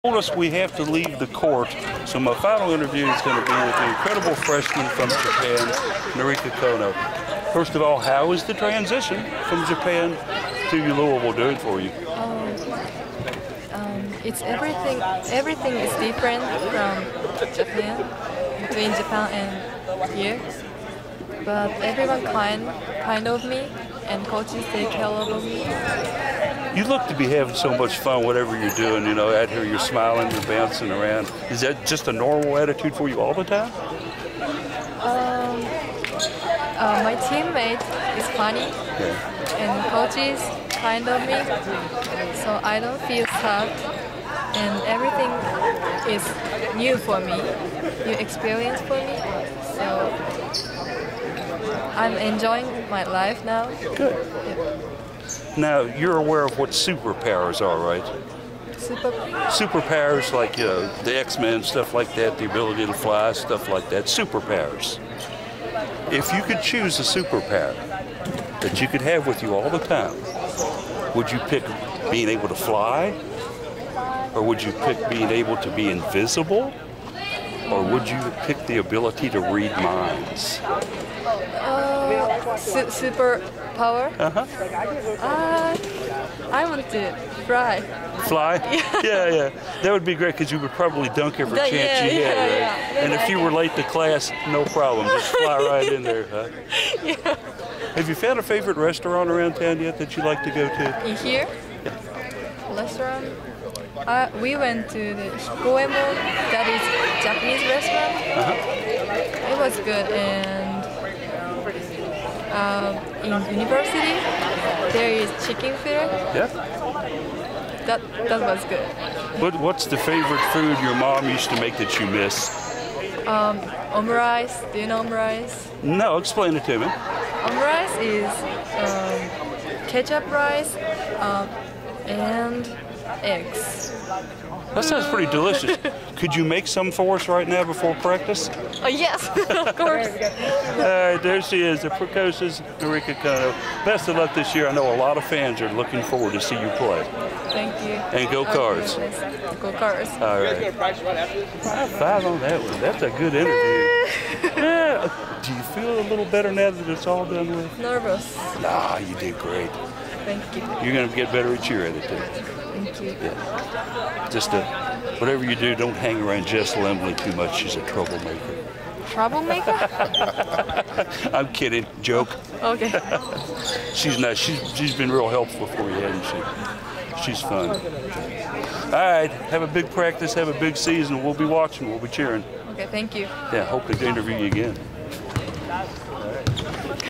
Louisville WBB Norika Konno - 2019 Media Day
- University of Louisville womens basketball's Norika Konno answers questions during Media Day 2019.